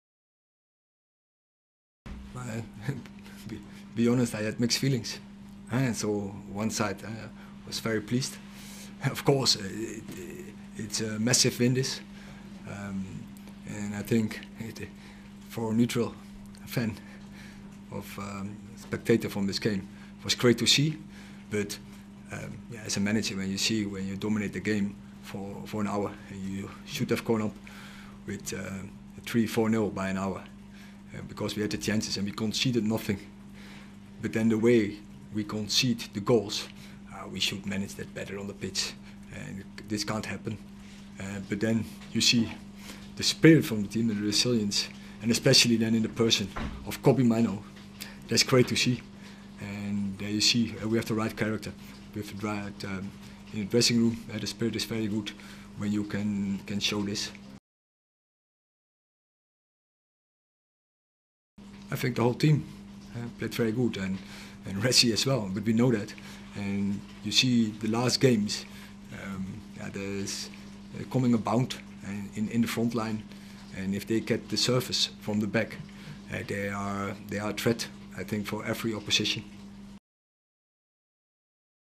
Ten Hag has 'mixed feelings' after Man United's dramatic 4-3 victory over Wolves
Manchester United manager Erik ten Hag faced the media following his side’s dramatic Thursday evening 4-3 win over Wolves at Molineux.